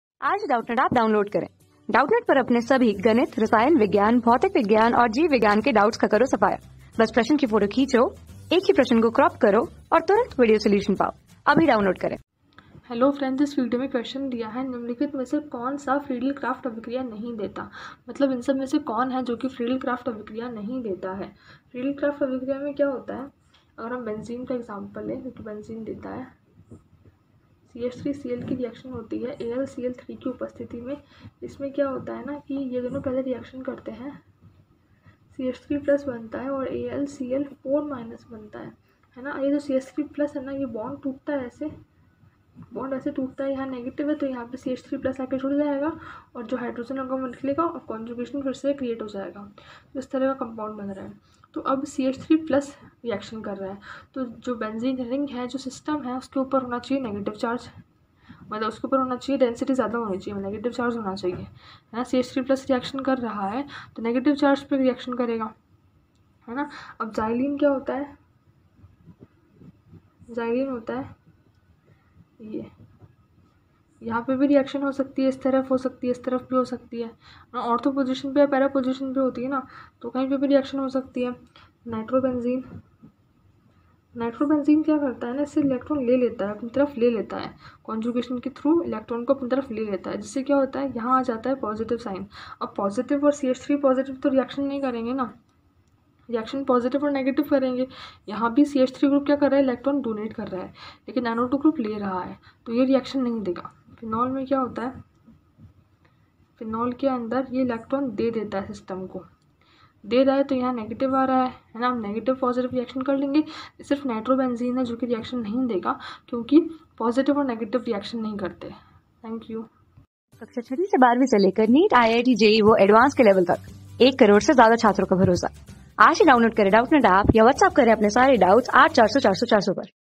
निम्नलिखित में से कौन-सा यौगिक फ्रीडल-क्राफ्ट अभिक्रिया नहीं देता है? | 12 | SOLVED PAPER 2010
निम्नलिखित में से कौन-सा यौगिक फ्रीडल-क्राफ्ट अभिक्रिया नहीं देता है?
Class: 12
Subject: CHEMISTRY
Chapter: SOLVED PAPER 2010
Board:UP Board

👉 Have Any Query? Ask Us.